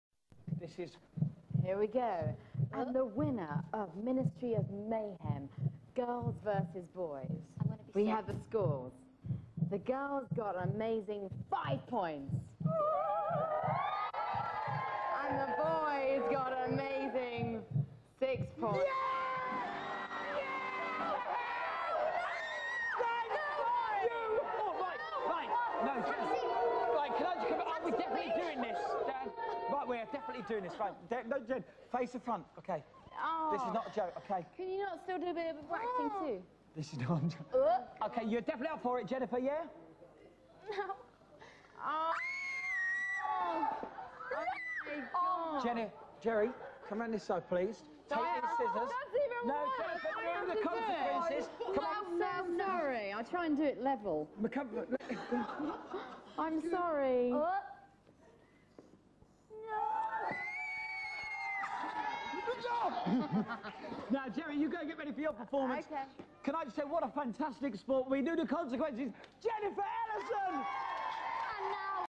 Spicegirl Barber
Spicegirl gives Jenifer Elison a surprise haircut.